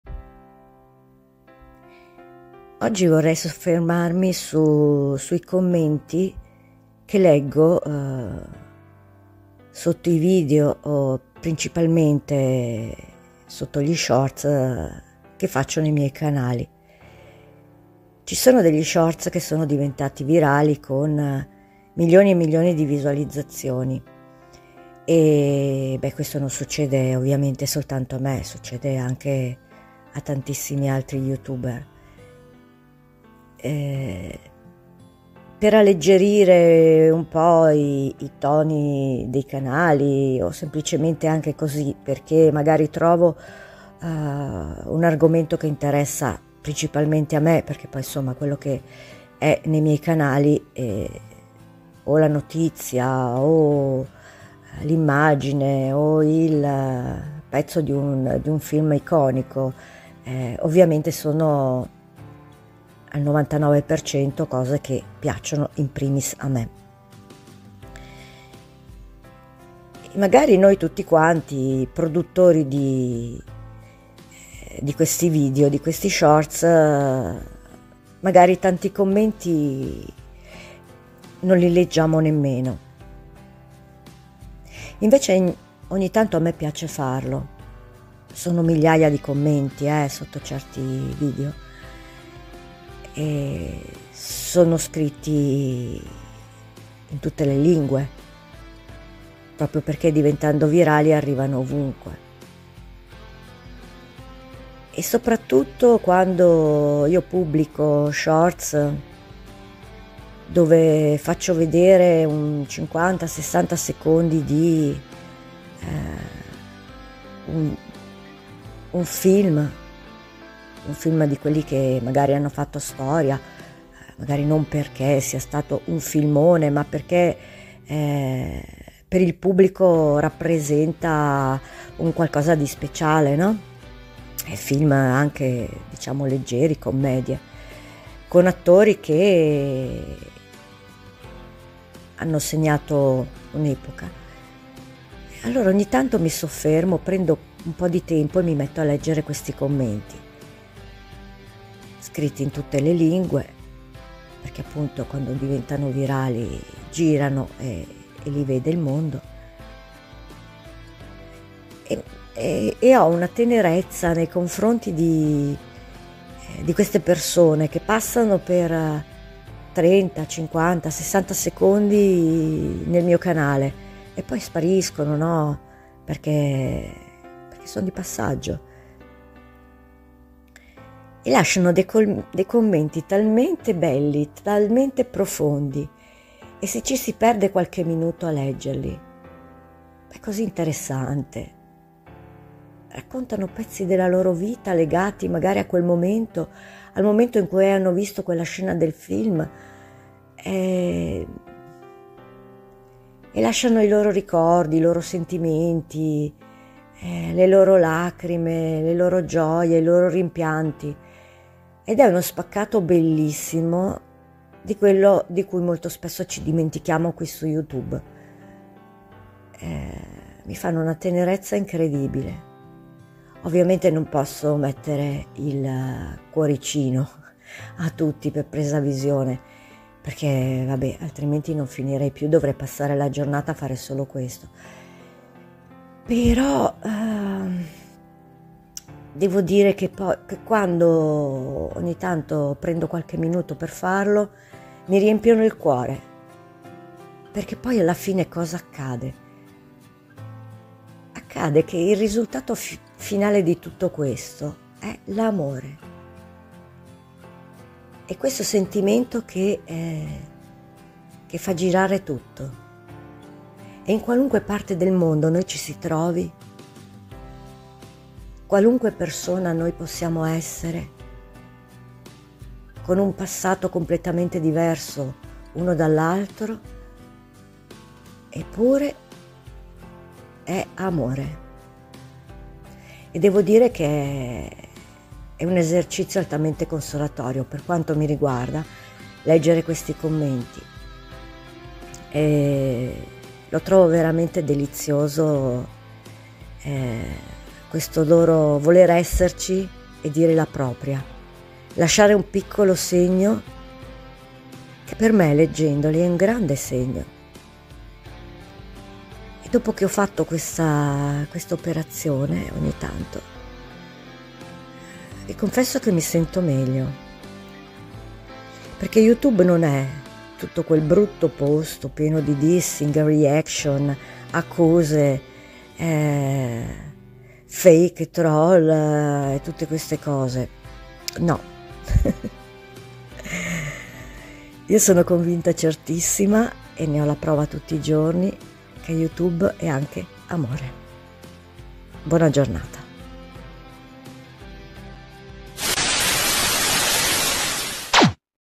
SU YOUTUBE VINCE CHI AMA 💓 [ENG SUB]
Grazie a Tutti Voi!
Un enorme grazie a tutti voi che lasciate un commento sotto i miei video. La vostra partecipazione è fondamentale e dimostra che YouTube non è solo una piattaforma di critiche e negatività. La vostra presenza crea una comunità vera e partecipe. Sono davvero felice di vedere quanto possiamo costruire insieme!

A huge thank you to all of you who have commented under my videos. Your participation is very important and it shows that YouTube is not a platform where criticism and negativity are necessarily prominent. Your presence creates a real close-knit community. I am really glad to see how much we are building together!